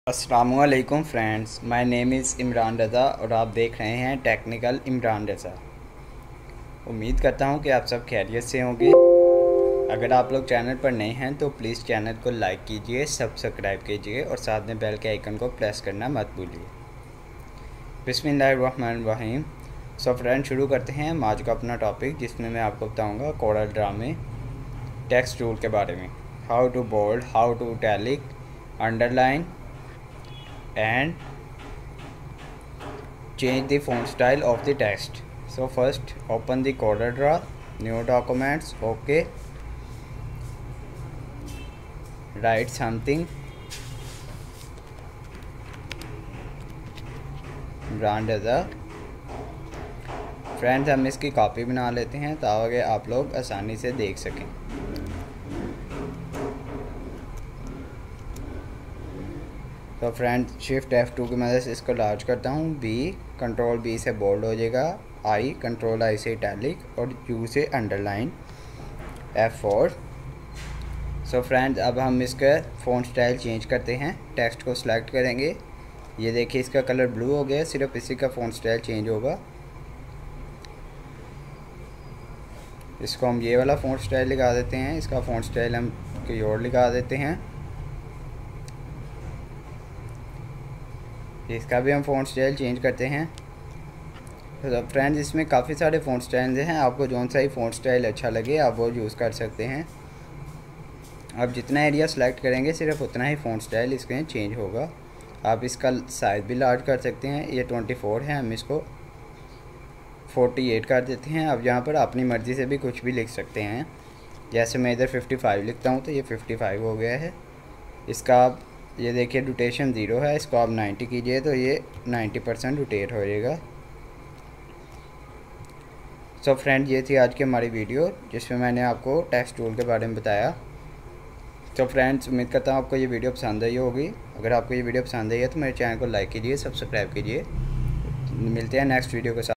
CoralDraw 2019 (Font, Italic, Underline, Bold)
I this video viewers will know how to write, bold the letter, italic the letter, and underline the letter and the word.
this is very useful video for the beginners.